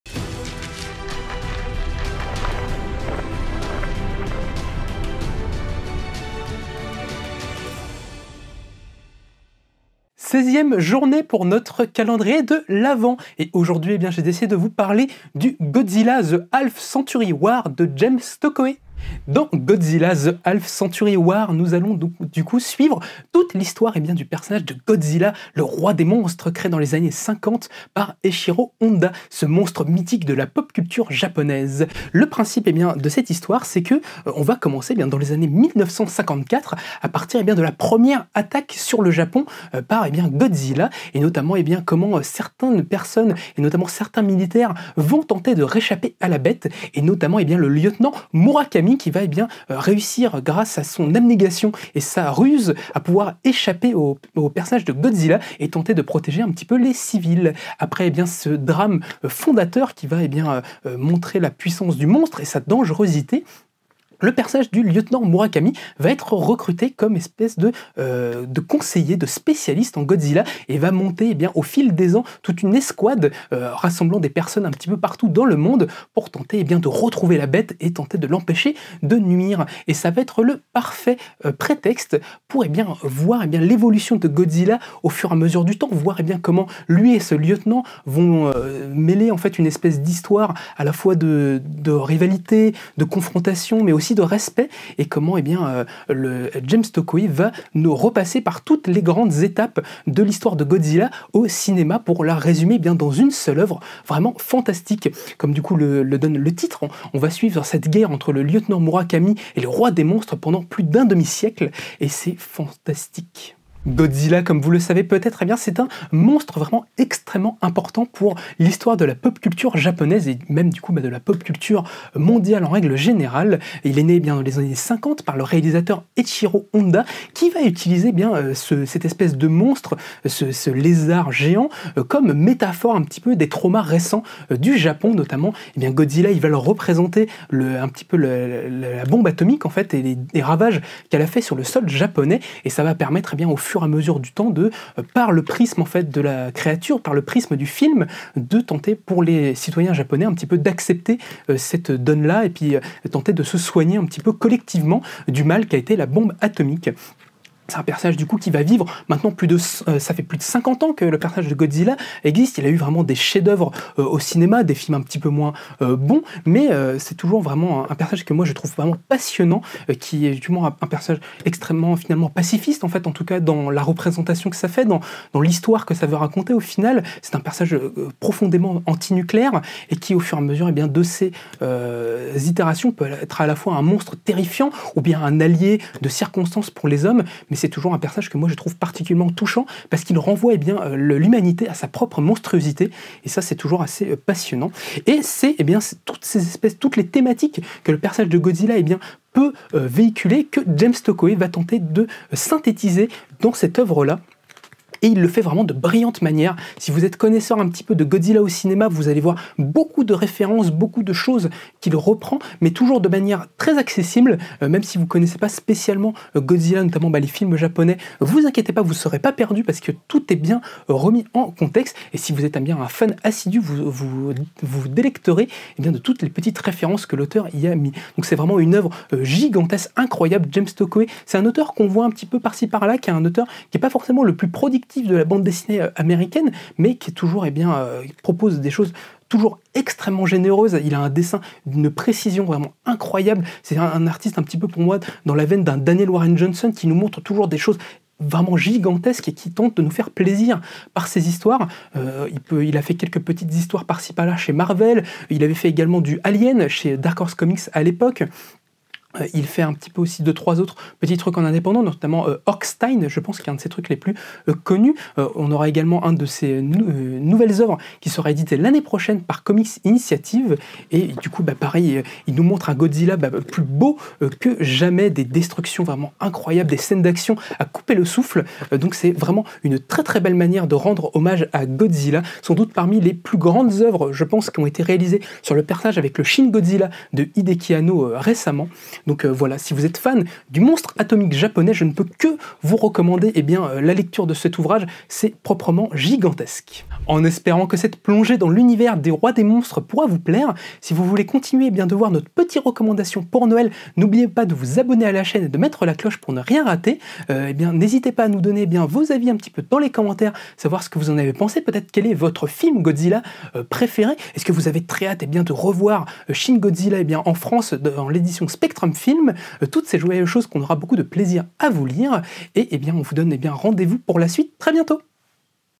Calendrier de l'Avent #16 - Godzilla The Half Century War de James Stokoe ☣️
Bienvenue chez Original Manga pour ce seizième numéro de notre Calendrier de l'Avent 2022 !

Aujourd'hui, Quentin vous présente Godzilla The Half Century War de James Stokoe, le plus bel hommage au grand lézard japonais 📖